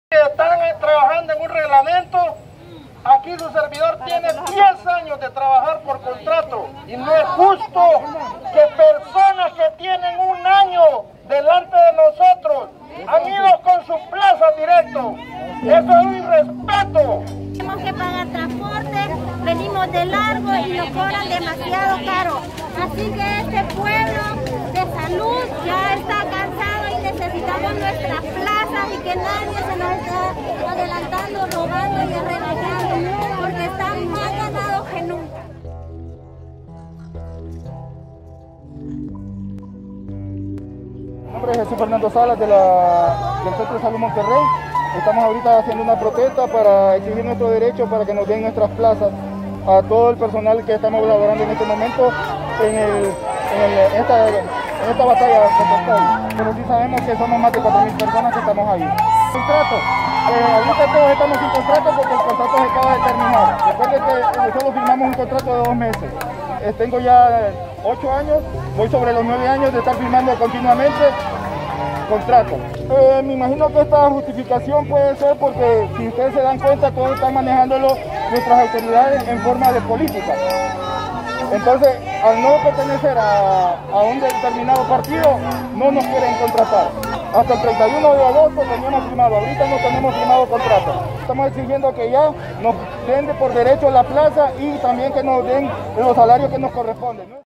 Empleados de salud protestan frente a Secretaría de Finanzas por falta de pago de salarios
Empleados del sector salud, permanentes y por contrato, protestaron por el imcumplimiento de la Ley de Contratación para el personal de emergencias y para el personal bajo la modalidad de contrato.